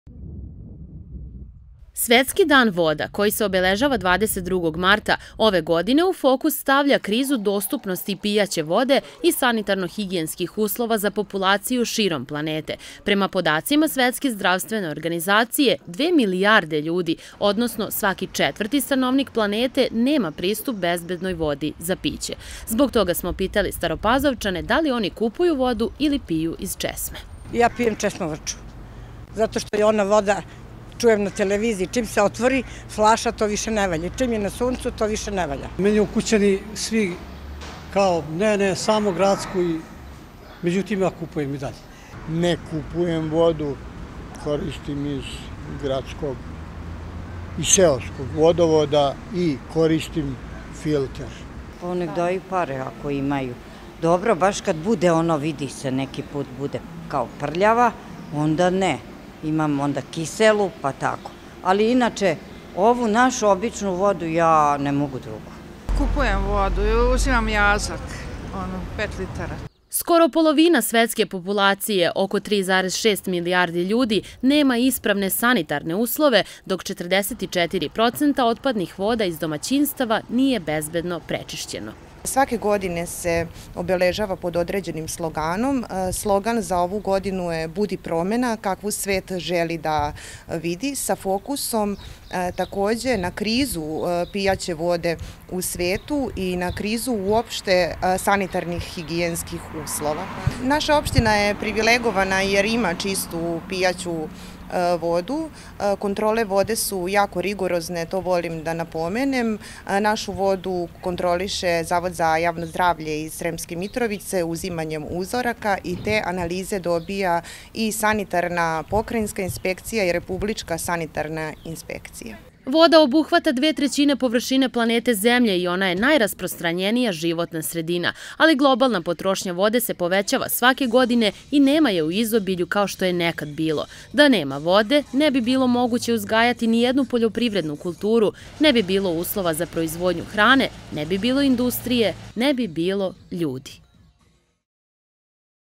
22. Mart svetski dan voda
Danas se širom sveta obležava Svetski dan voda. Nažalost za mnoge stvari u životu shvatimo koliko su važne, tek kada već bude kasno – tek kada ih izgubimo. Pa tako mnogi vodu uzimaju zdravo za gotovo, jer im je lako dostupna i imaju je u izobilju.